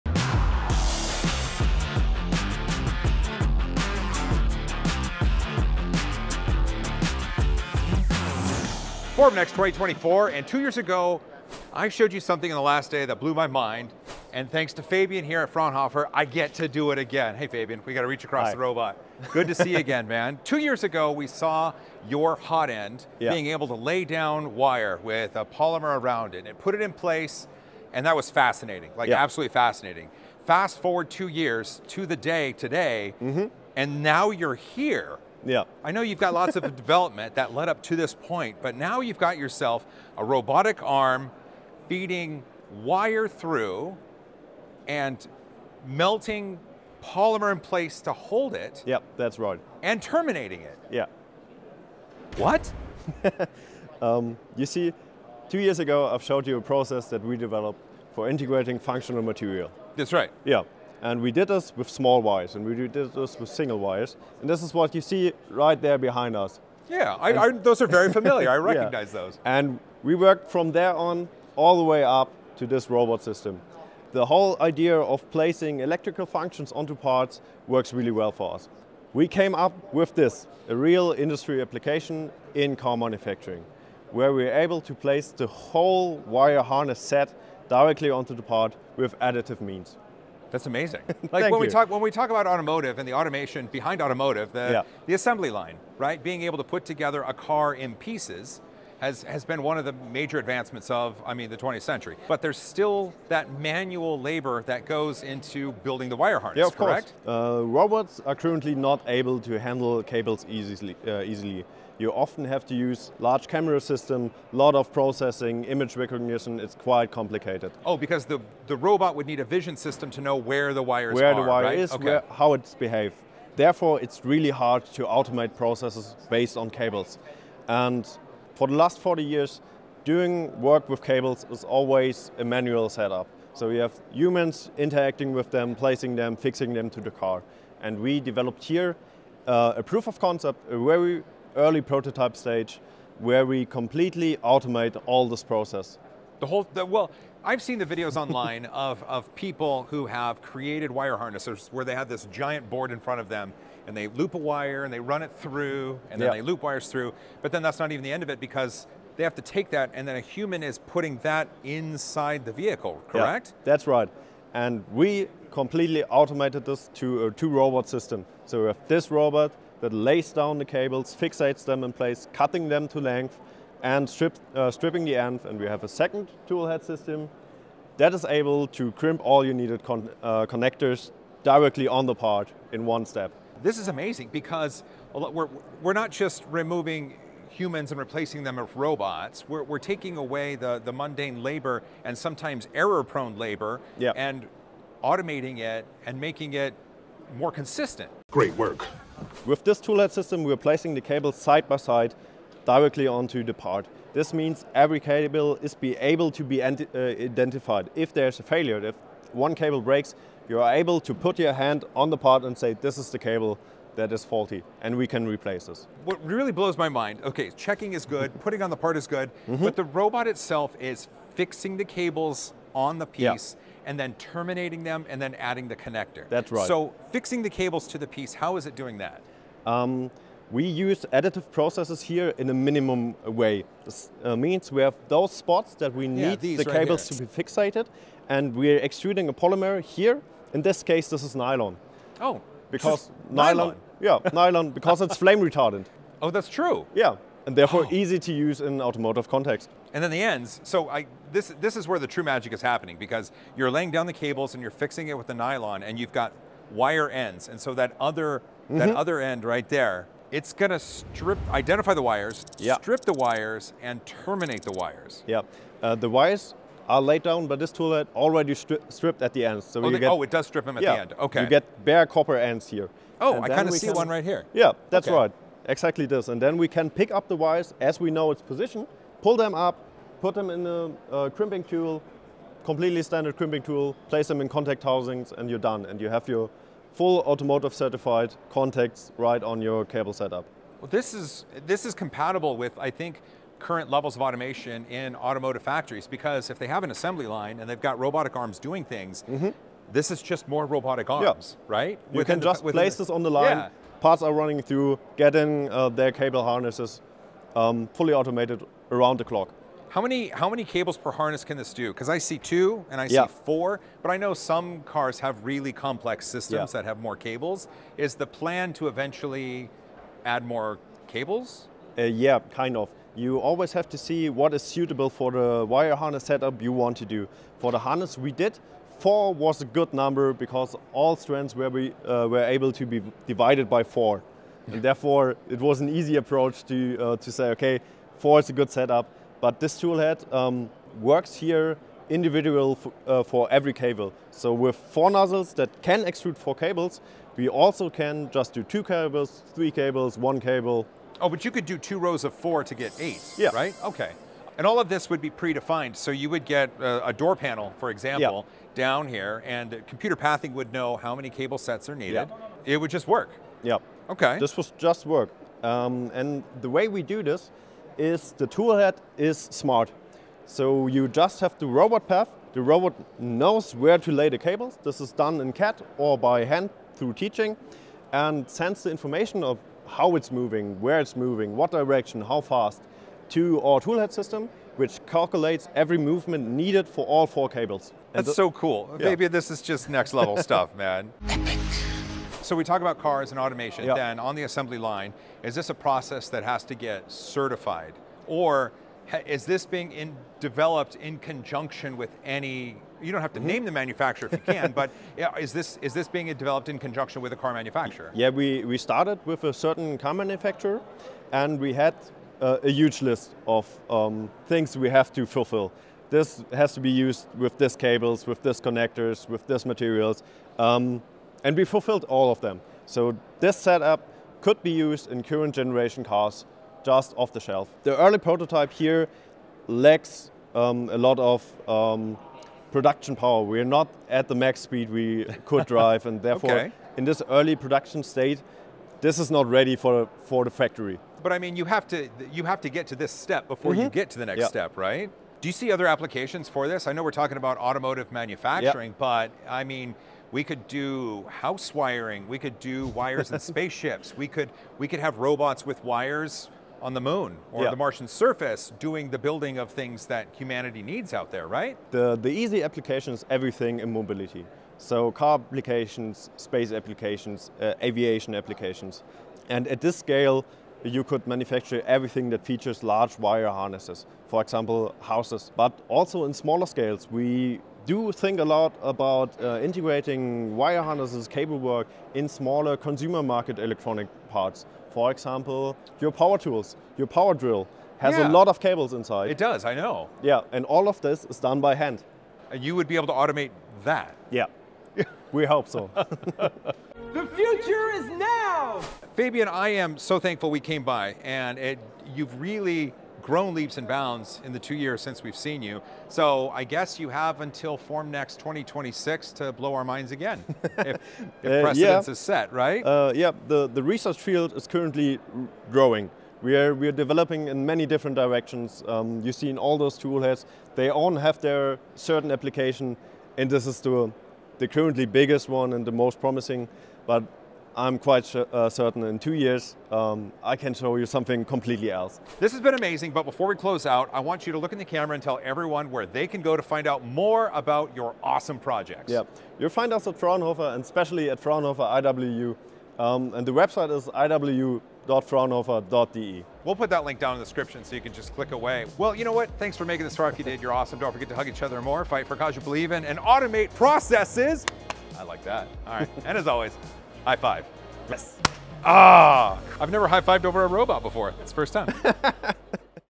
3D PRINTING WIRE and TERMINATING IT?! - Fraunhofer Formnext 2024
Fraunhofer has blown my mind yet again, with a robitic arm that 3d prints wire in wire looms, then terminates the wires, and uses Nylon to hold the wires in place. MADNESS.

THE TEAM! 🤟

Find Me Socially!

Want to send me something?
3D Printing Nerd
USA